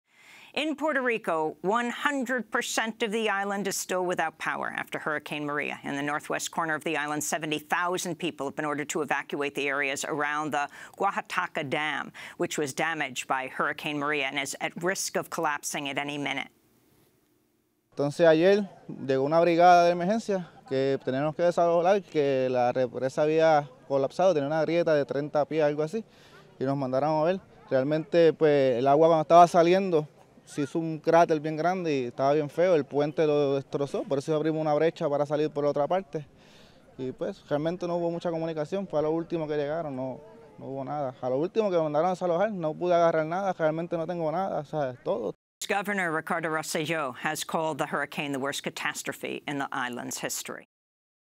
Puerto Rico: miles de personas evacuan por el posible colapso de una represa tras el huracán María
El 100% de la isla sigue sin electricidad tras el huracán María. En la región noroeste, 70.000 personas han recibido órdenes de evacuar las áreas cercanas a la represa de Guajataca, que fue dañada por el huracán y está en riesgo de colapsar “en cualquier momento”. El gobernador de Puerto Rico, Ricardo Roselló, ha calificado el huracán de la peor catástrofe en la historia de la isla.